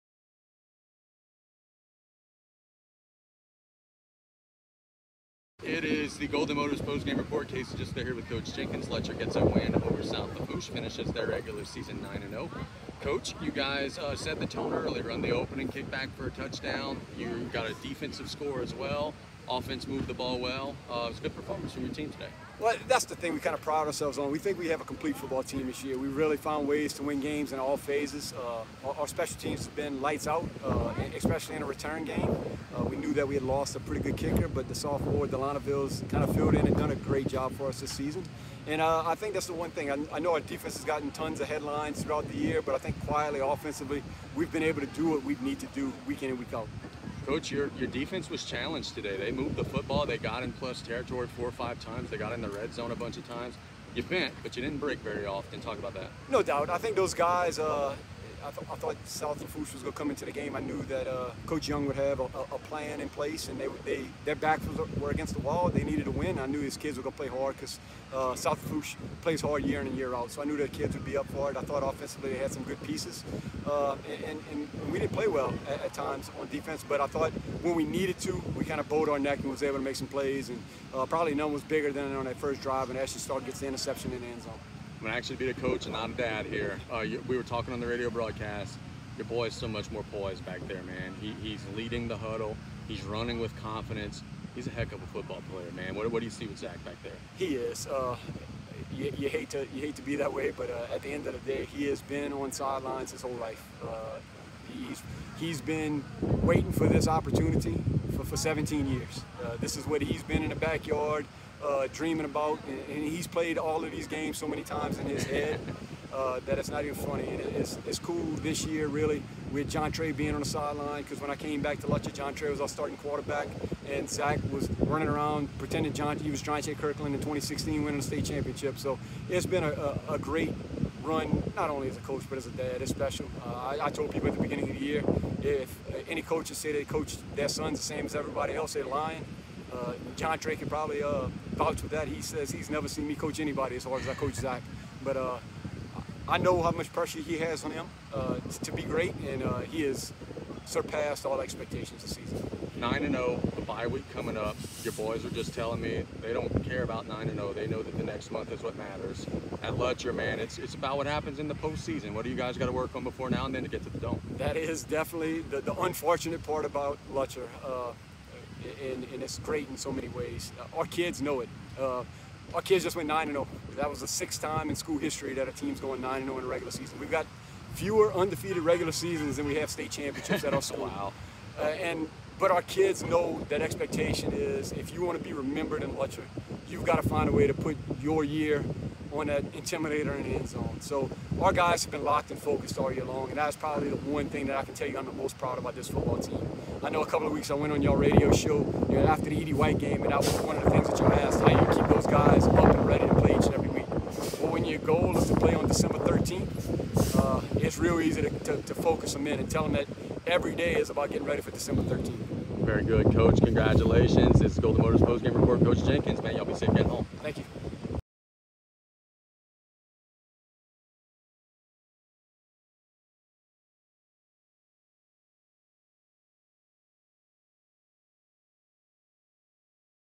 Golden Motors Postgame Report: Dwain Jenkins discusses win
Lutcher got a win over South Lafourche on Friday and coach Dwain Jenkins was happy with how his team started on the way to the win.

See his comments in this Golden Motors Postgame Report.